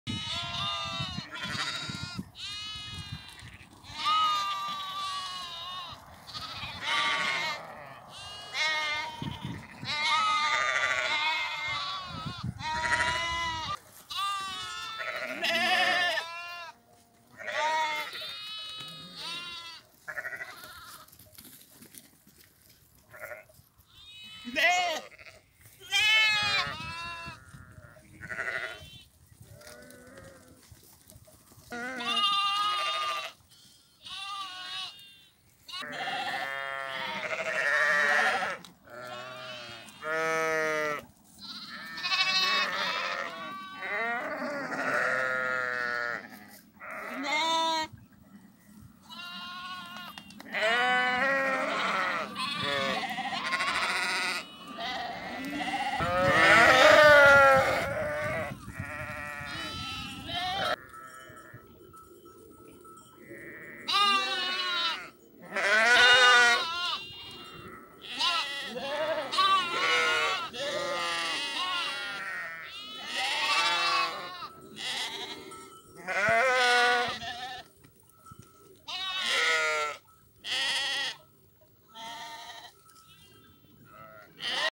Kuzuların keyfi yerinde
Baharın ilk günleri temiz hava bol güneş keyifler yerinde